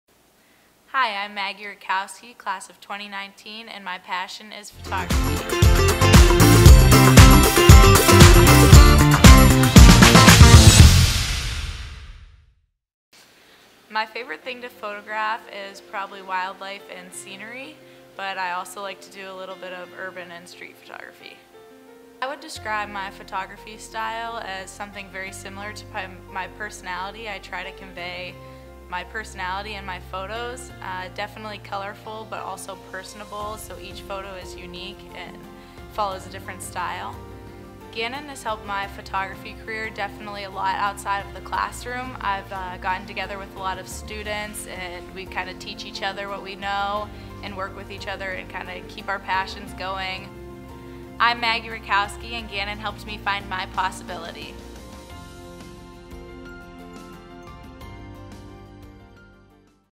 Maggie Rutkowski Photography | Edge Spotlight #3
A series designed to showcase past, present and soon to be Gannon University students who are doing incredible things!

Follow Maggie!